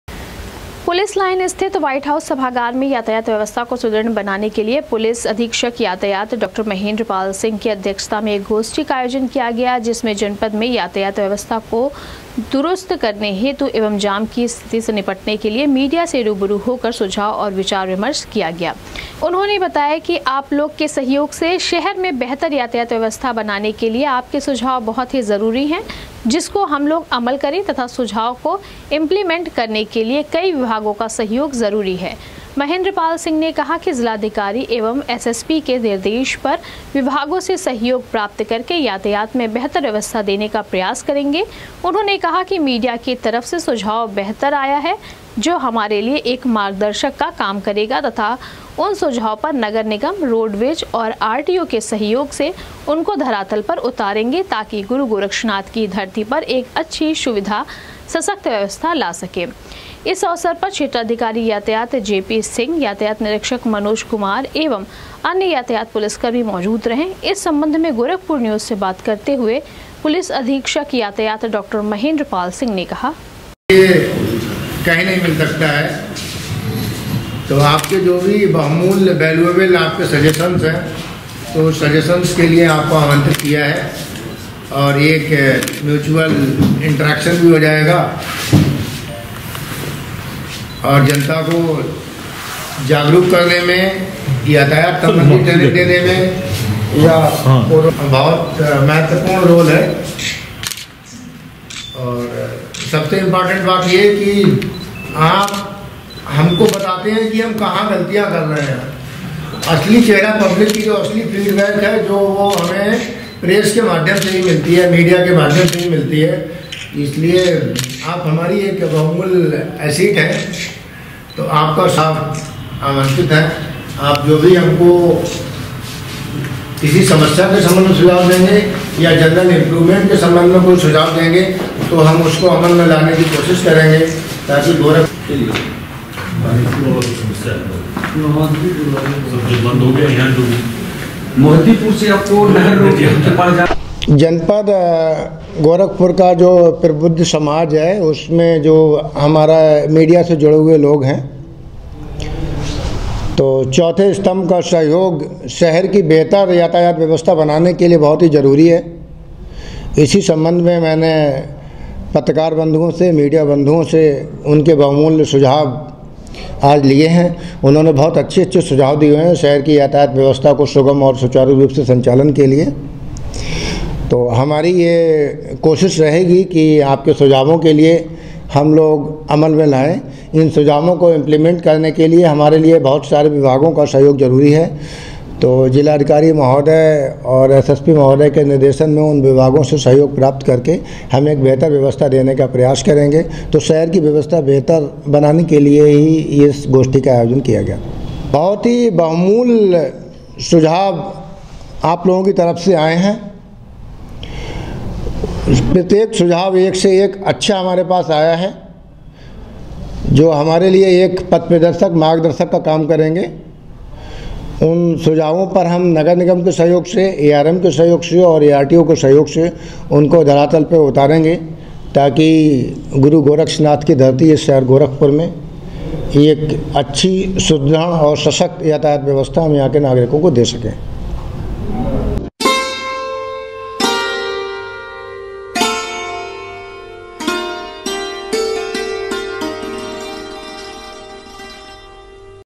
पुलिस अधीक्षक यातायात की अध्यक्षता में यातायात व्यवस्था को सुदृढ़ बनाने के लिए मीडिया के
पुलिस अधीक्षक यातायात की अध्यक्षता में यातायात व्यवस्था को सुदृढ़ बनाने के लिए मीडिया के साथ आयोजित किया गया गोष्ठी,कहा, मीडिया के तरफ से आये है बेहतरीन सुझाव, उनको उतारेंगे धरातल पर ।।